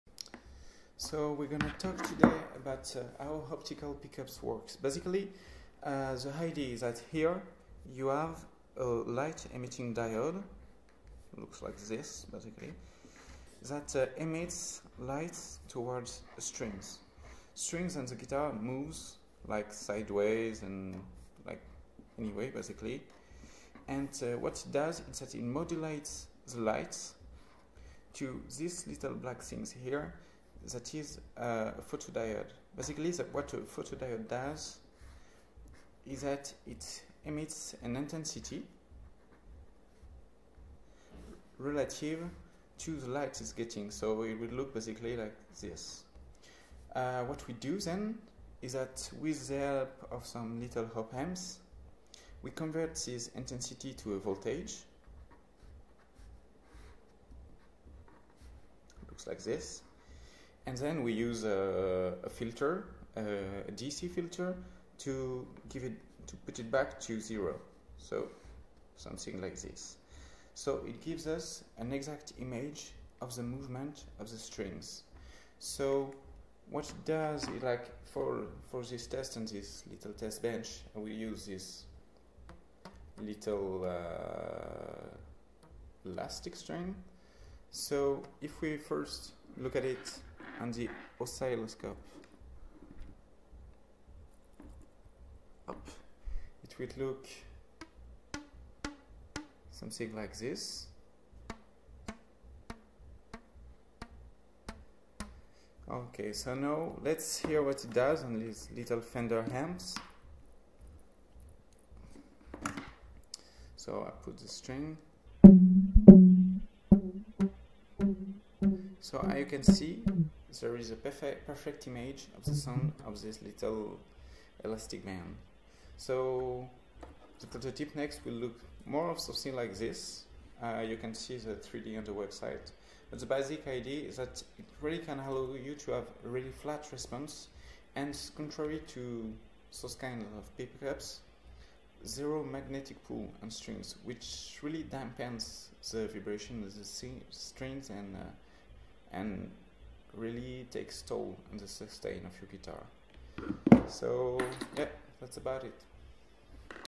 How do optical pickups work?
Her we describe how infrared optical pickups work